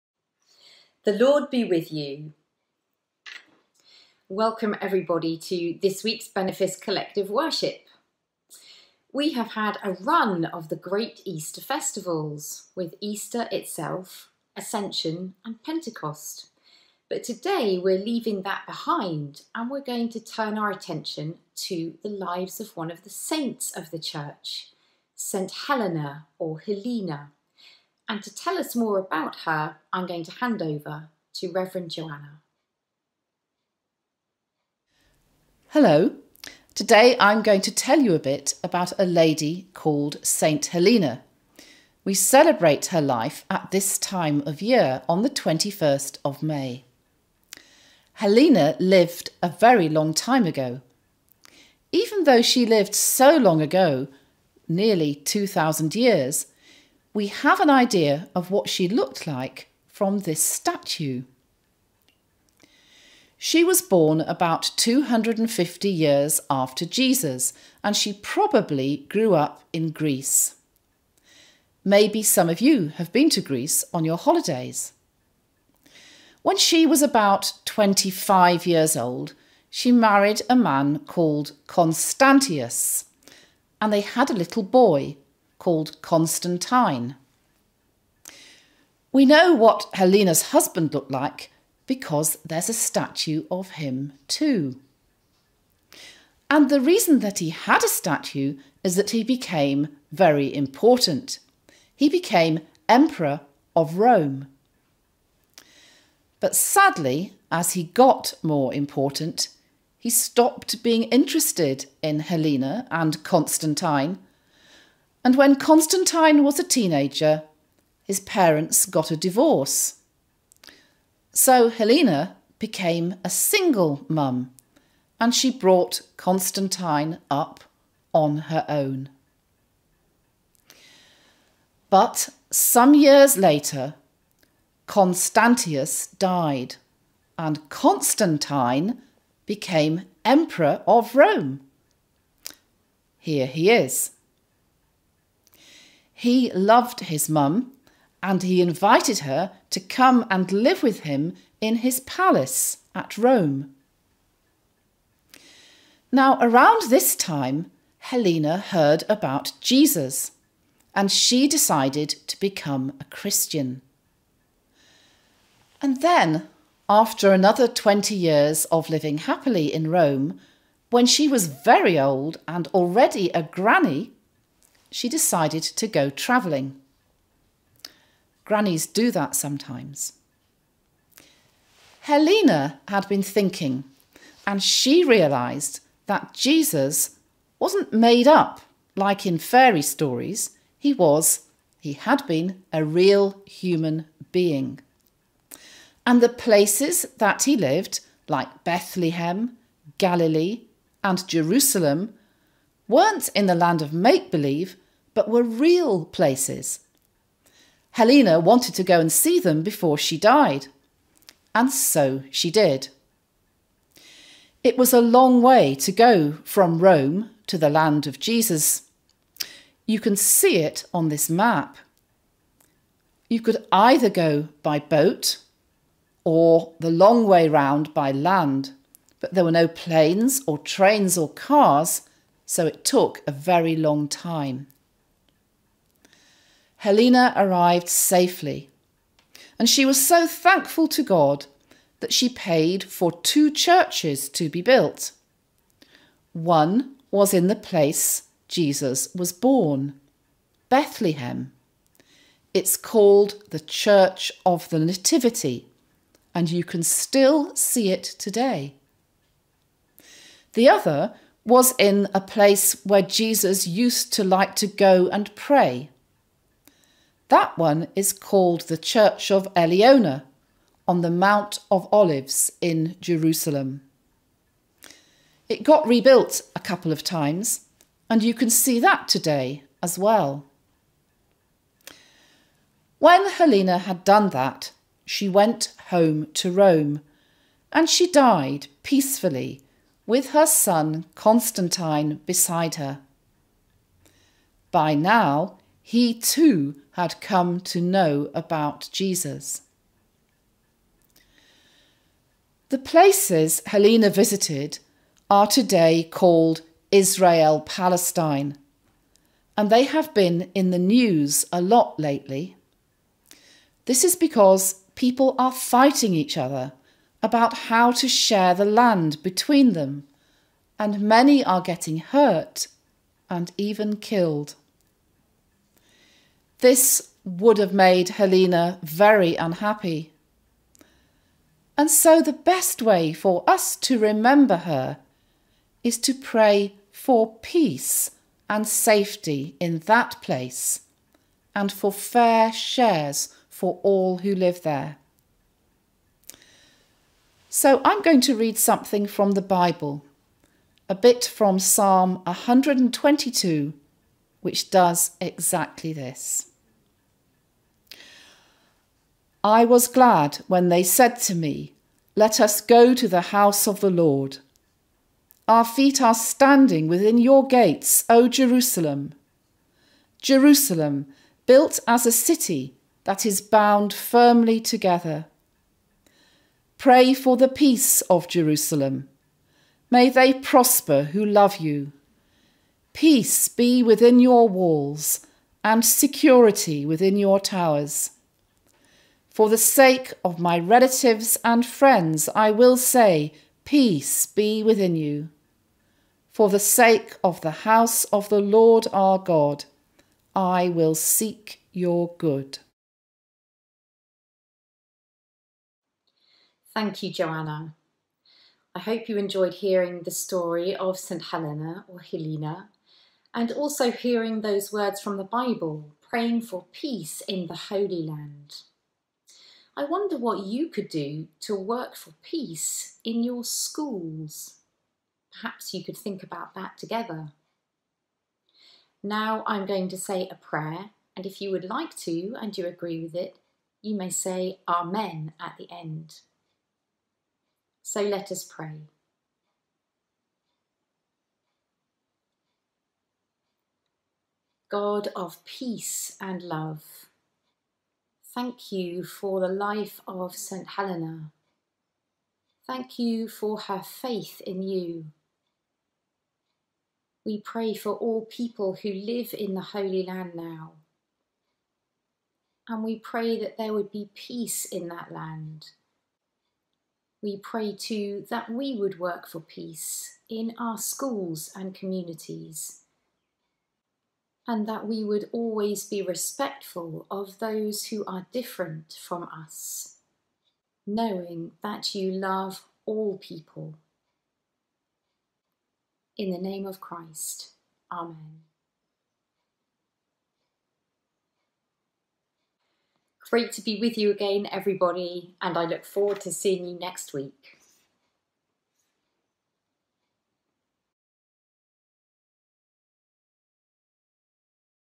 Rev'd Joanna Collicutt on St Helena
Joanna tells the story of St Helena and her pilgrimage to the Holy Land